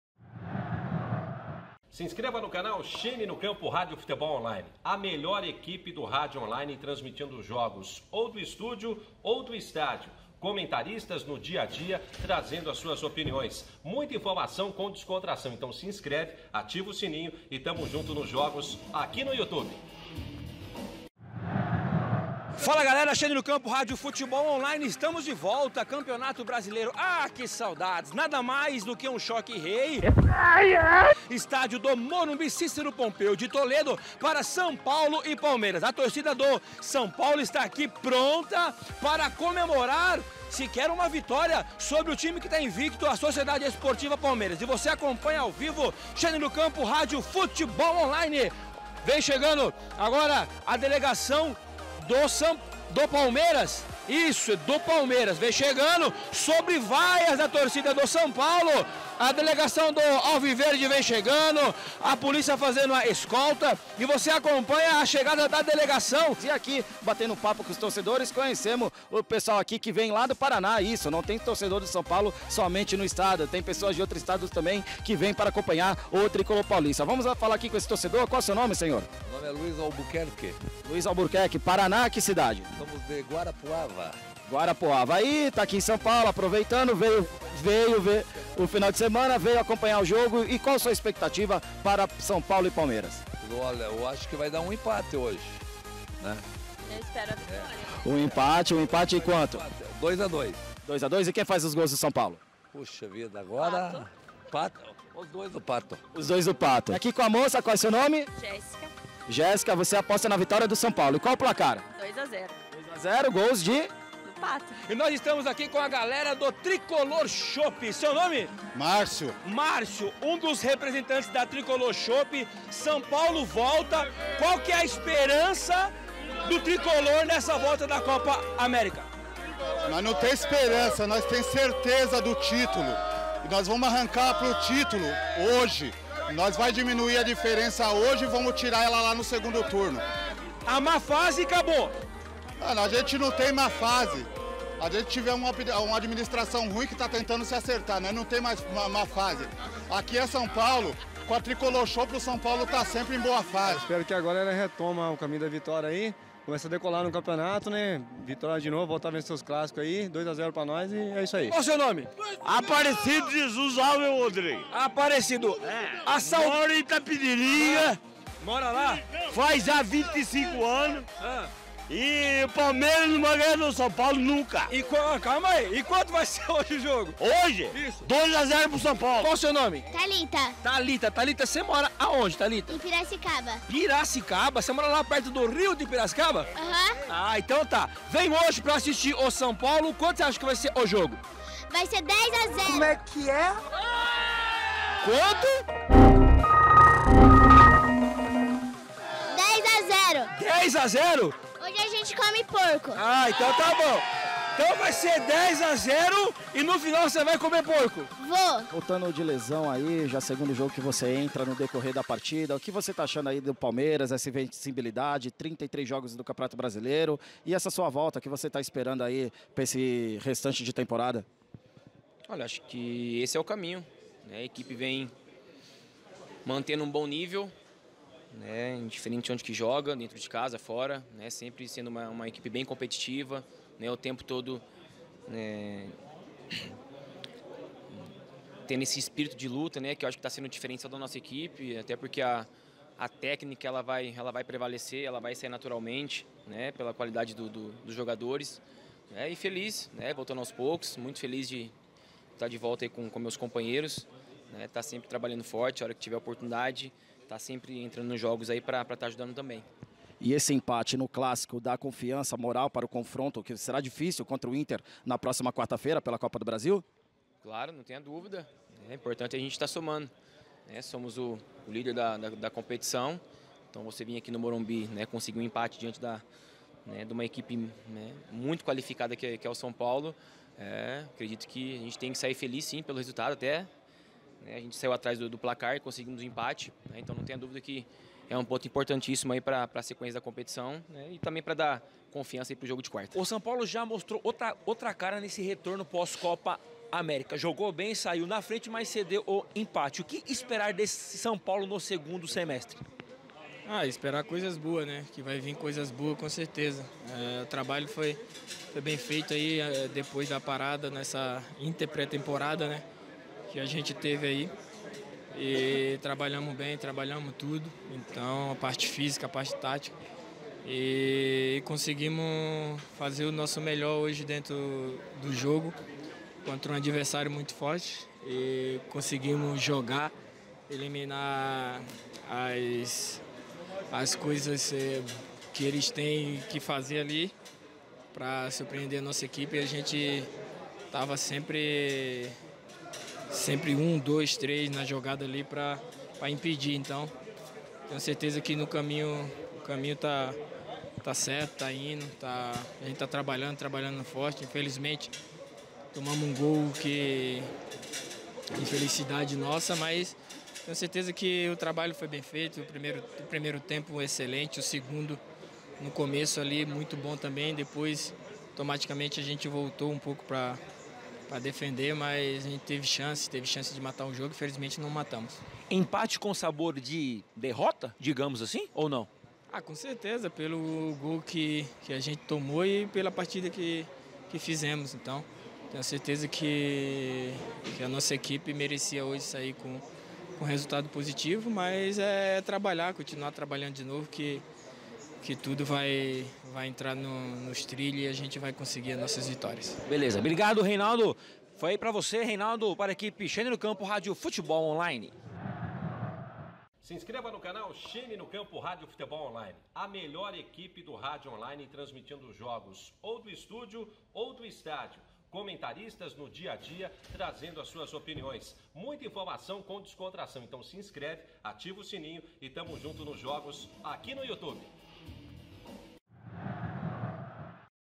Reportagem de São Paulo 1 x 1 Palmeiras - Campeonato Brasileiro 2019
Neste último sábado a equipe Cheni no Campo_RFO esteve presente do estádio do Morumbi no clássico Choque Rei que ficou no empate de 1 x 1, confira o pré jogo, entrevista com a torcida e pós jogo. Campeonato Brasileiro estavamos com saudades, VOLTAMOS!!